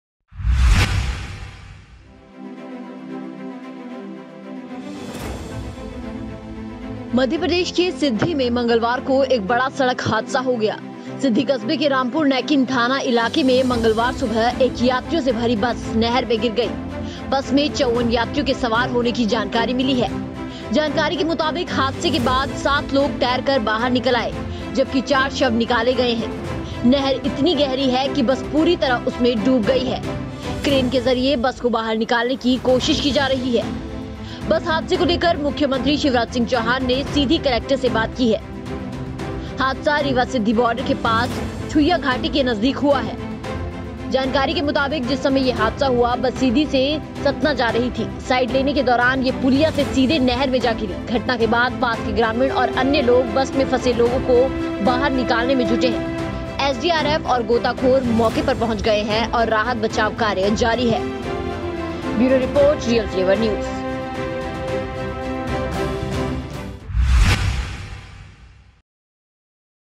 मध्यप्रदेश में बड़ा सड़क हादसा
यात्रियों से भरी बस नहर में गिरी
54 लोग थे सवार